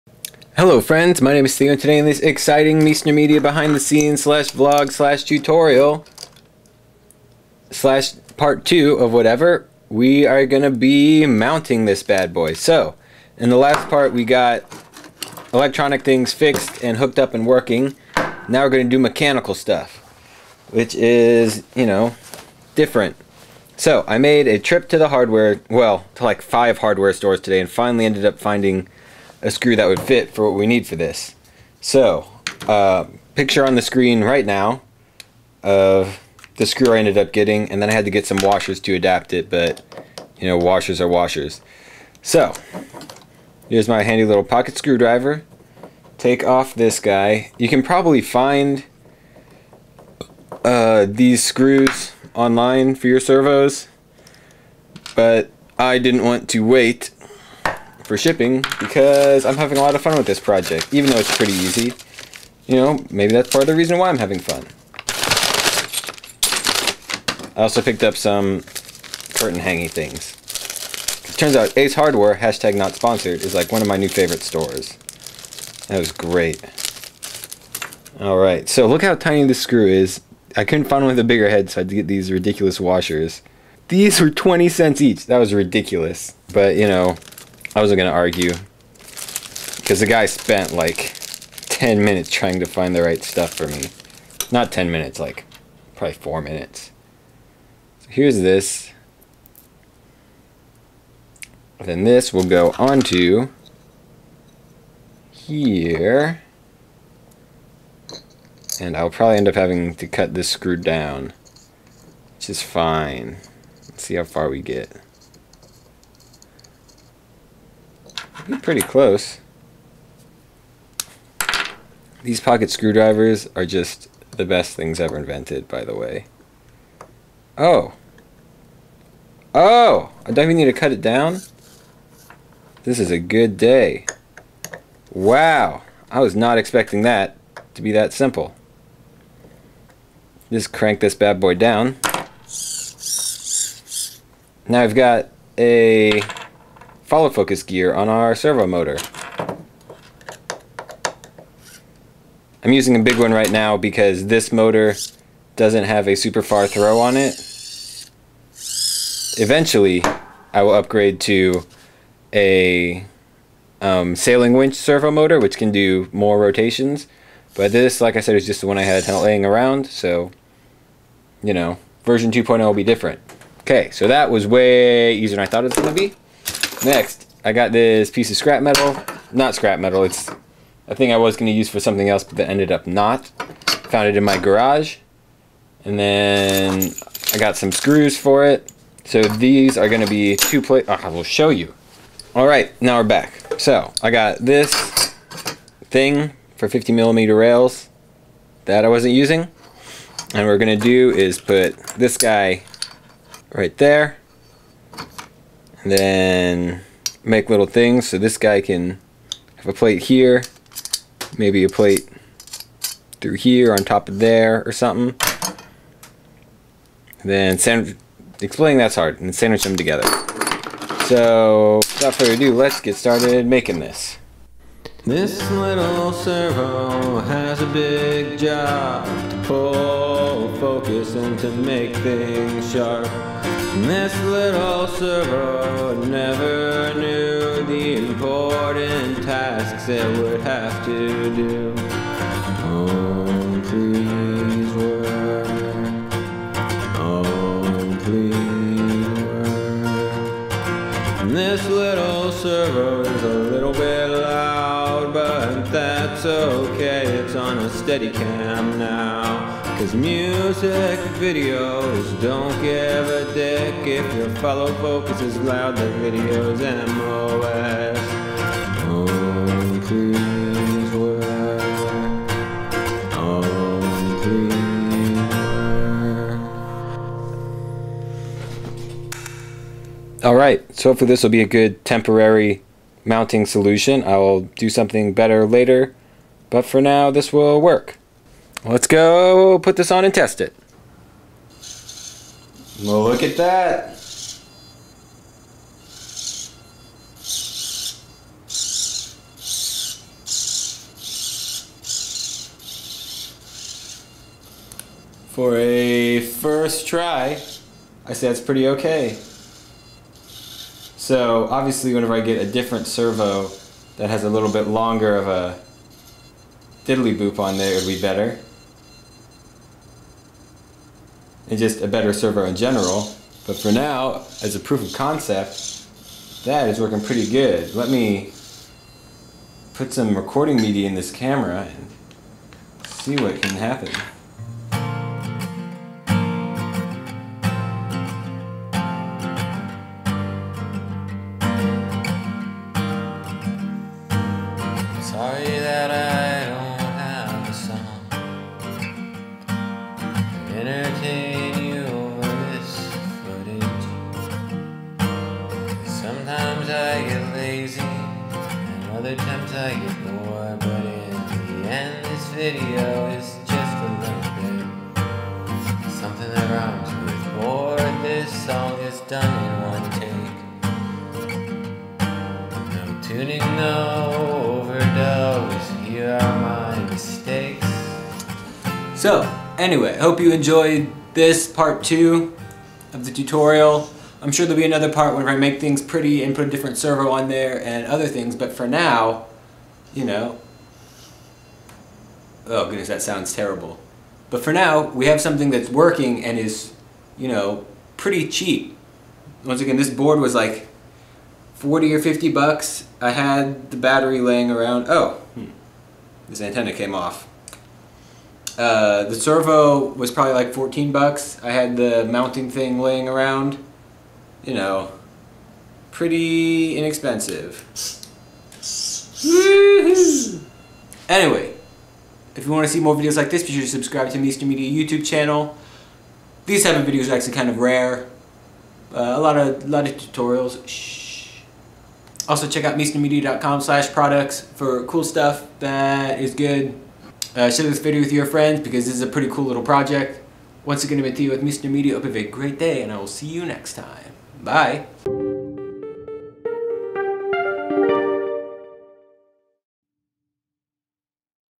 SUPER CHEAP DIY WIRELESS FOLLOW FOCUS - Part 2
In this exciting installment of the DIY follow focus project we get the version 1 functional! All thats left is to make it prettier and better!